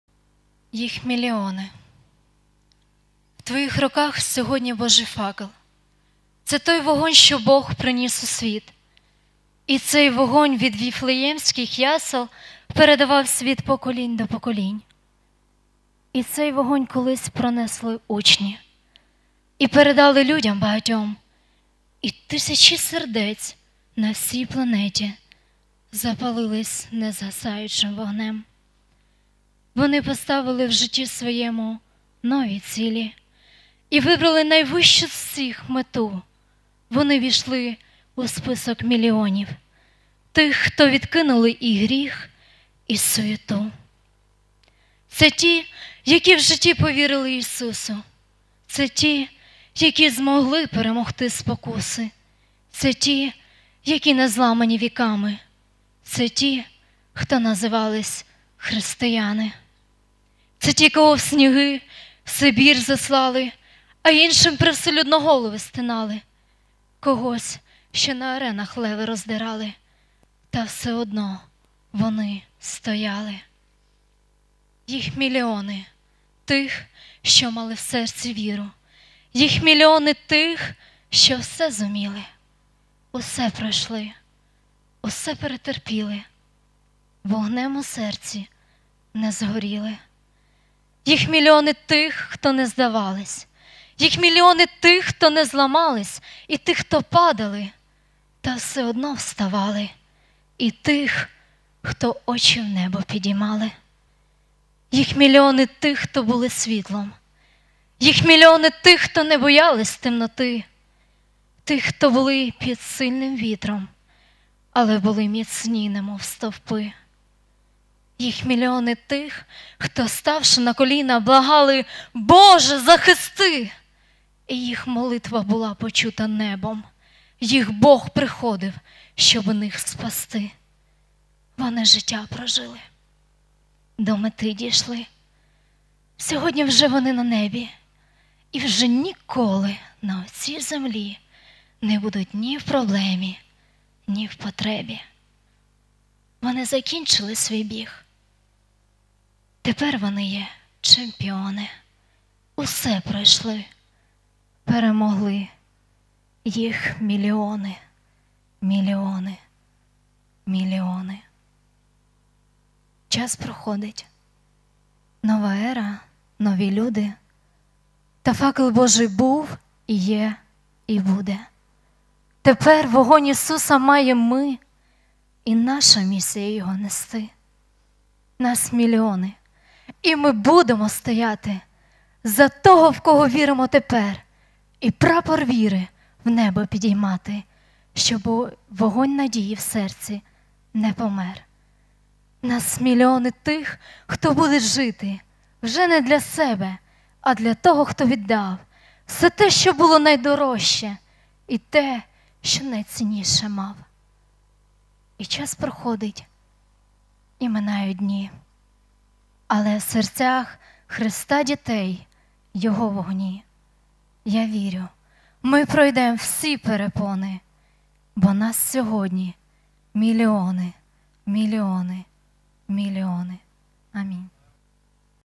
Їх міліони
Вечернее Богослужение 20.08.17, в церкви на Карьерной, 44 – стих, Вера Мовчан